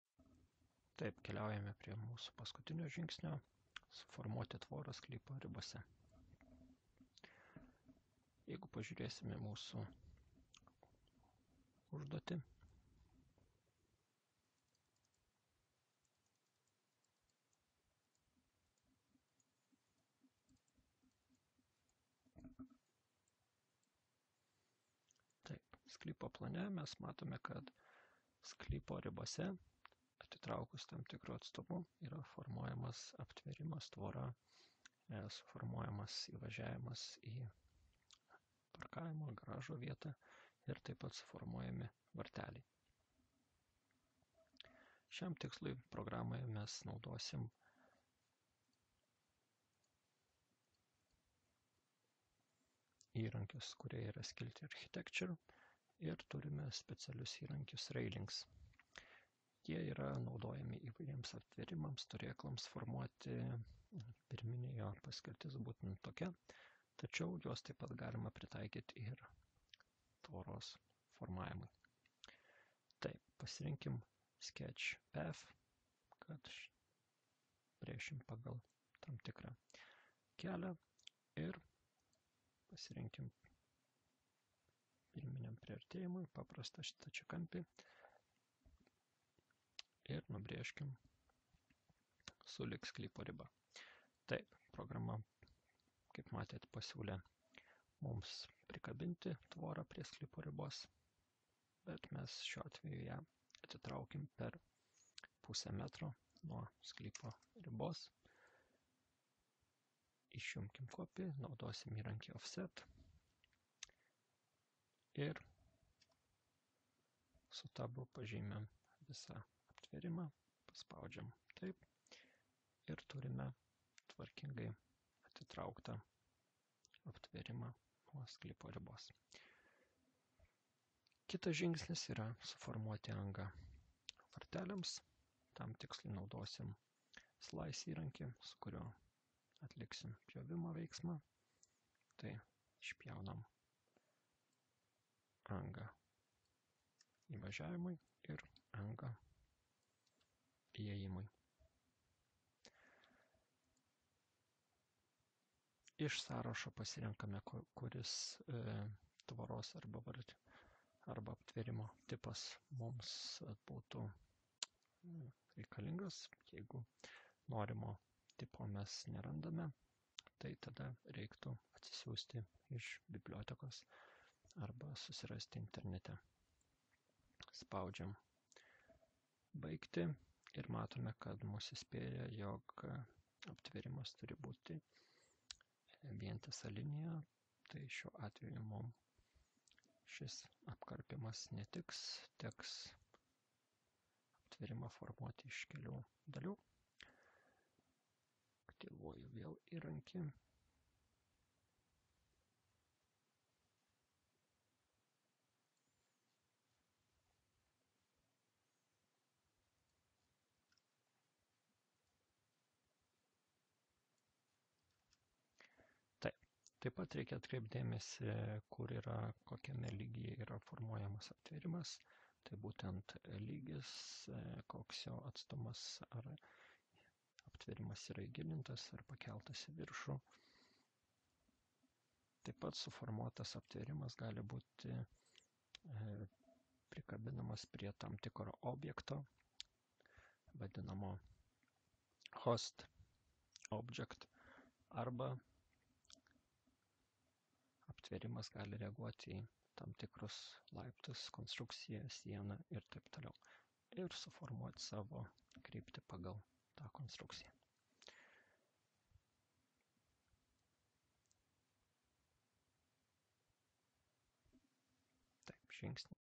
0122-1 LIT RevitTraining Revit Circulation Railing (tvora)
Products
Version: 2019
Language: Lithuanian
Skill Level
Category

In this Revit training video, we will guide you through the final steps of forming a fence within a property boundary. Learn how to utilize Revit's powerful tools to sketch, adjust, and complete a professional fence design. This tutorial is perfect for architects and BIM enthusiasts looking to enhance their Revit skills.

👷 What you'll learn:
 • How to form a fence within a property boundary in Revit
 • Using Revit's architectural tools for fence formation
 • Adjusting and positioning fences with the offset tool
 • Creating and positioning gate openings in Revit
 • Selecting and completing different types of fences in Revit

00:00 - Introduction to Forming a Fence
00:03 - Overview of the Task
00:12 - Sketching the Fence Boundary
00:36 - Forming the Fence Structure
01:00 - Adjusting Fence Position with Offset Tool
01:28 - Creating Gate Openings
02:10 - Selecting Fence Types
02:32 - Completing the Fence Formation
03:00 - Final Adjustments and Considerations